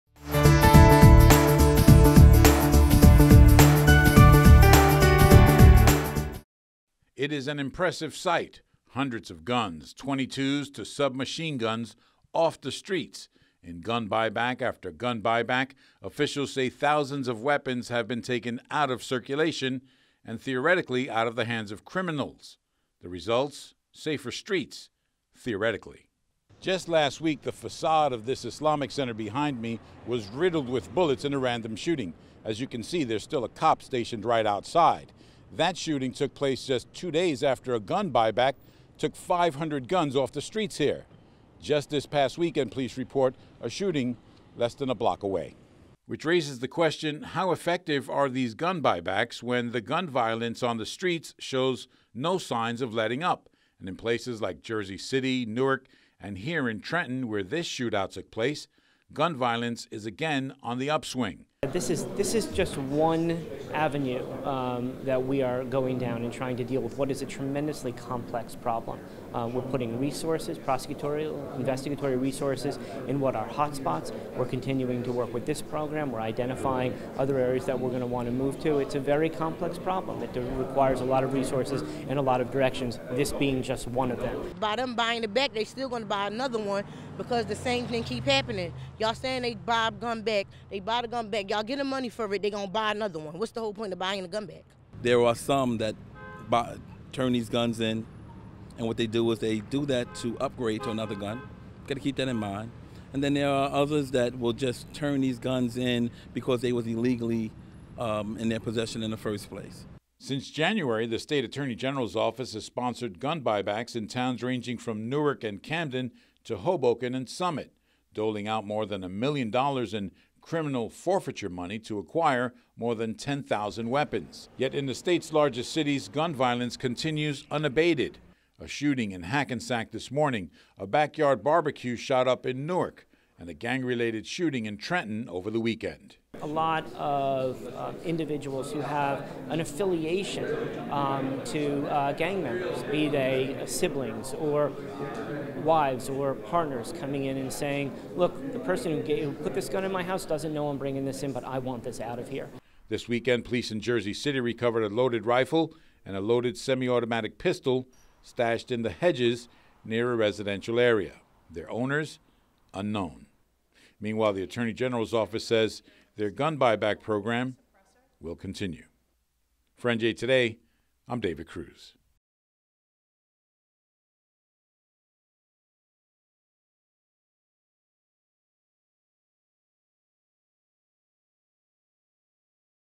Buybacks Get Thousands of Guns Off the Streets, Yet Shootings Still Continue
Despite gun buybacks taking thousands of weapons off the street, gun violence persists in many of the state's cities. For more New Jersey news, visit NJ Today